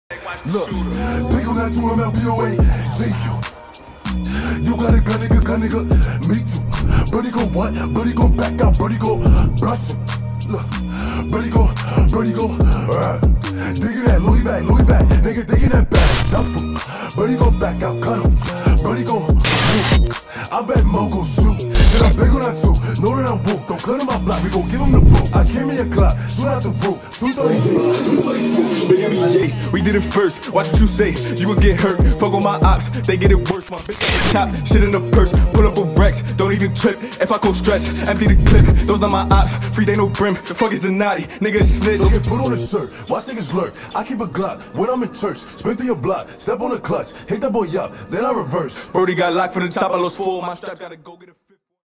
Proton Wira 1.6 Turbo Cinematic Shoot
Quarantine led to this small cinematic shoot.